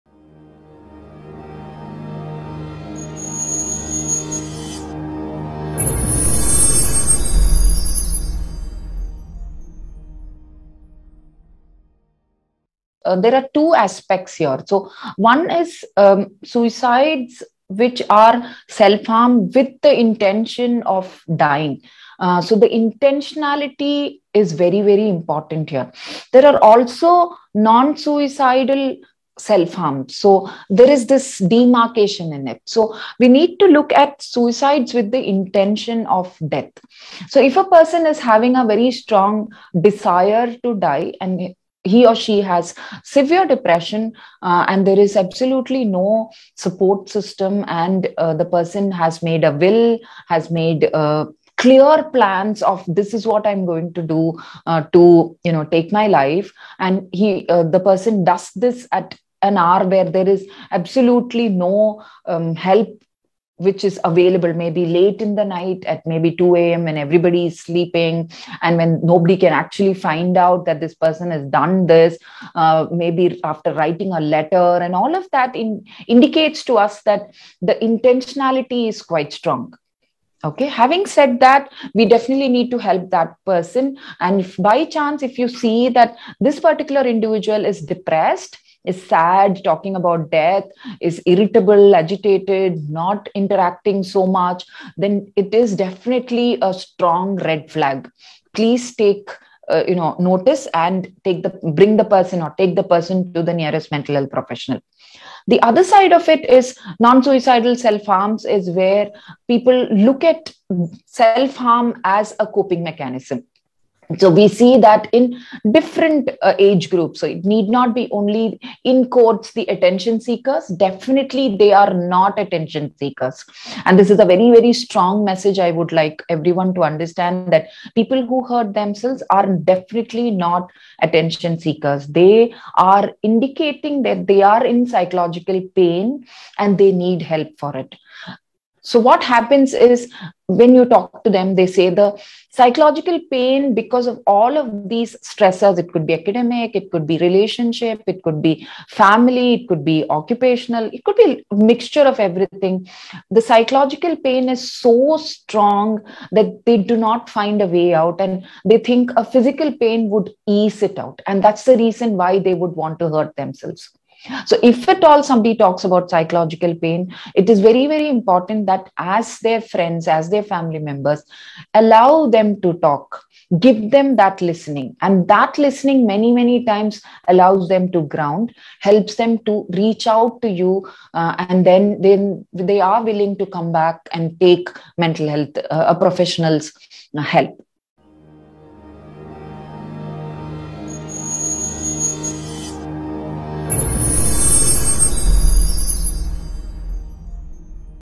Apollo Hospitals | Can mental stress lead to suicidal thoughts and self harm? | Dr Keerthi Pai
Although selfinjury is not usually a suicide attempt, it can increase the risk of suicide because of the emotional problems that trigger self-injury. And the pattern of damaging the body in times of distress can make suicide more likely.